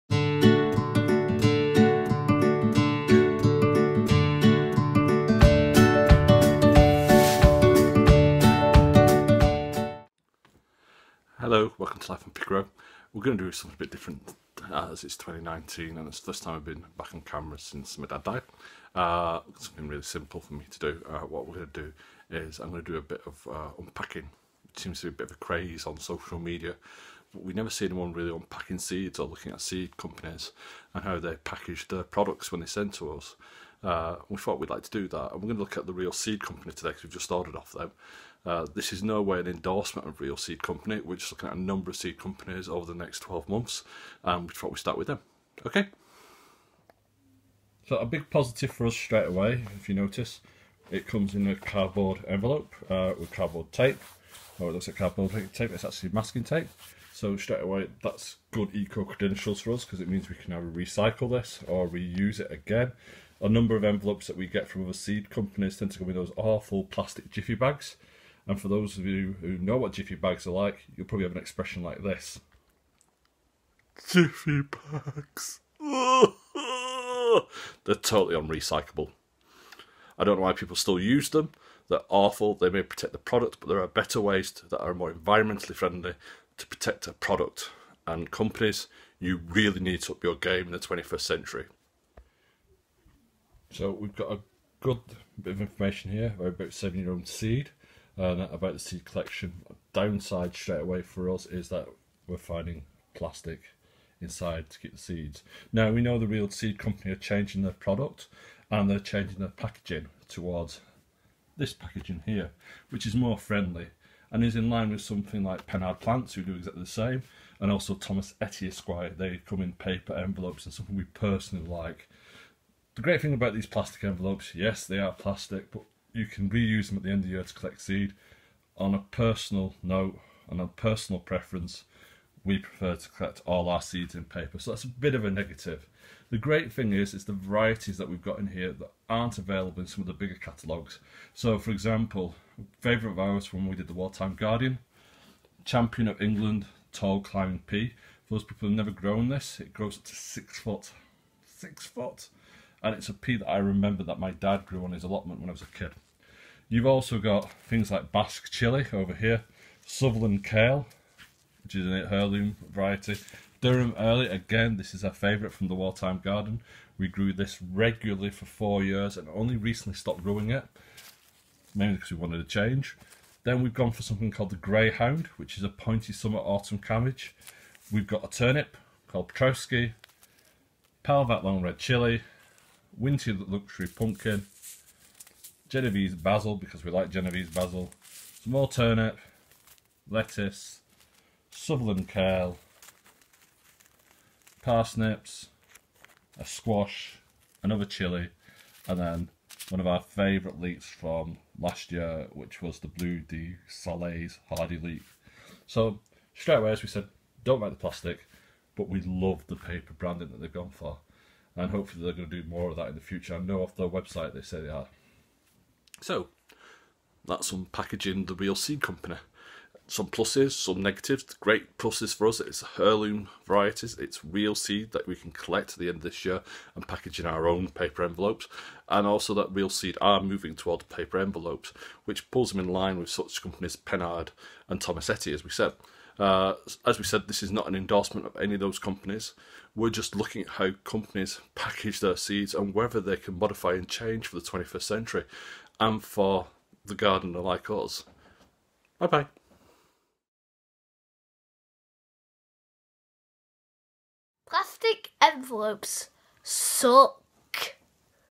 Unboxing: Real Seeds
Unboxing different seed companies to see whether they can meet the environmental needs of the twenty-first century. These films do not endorse the seed companies discussed but look at their packaging practice. All seeds were purchased by us and the companies/organisations looked at were unaware that we were doing this. This has been done to receive products that every gardener receives. Come unbox some seeds, it's green to grow but how green is it to receive seeds through the post?